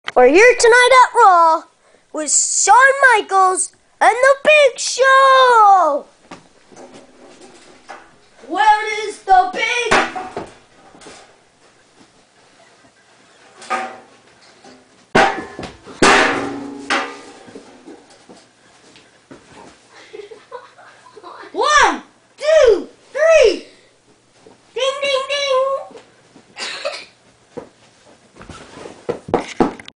shawn michaels vs. big show
shawn michaels hits big show with a chair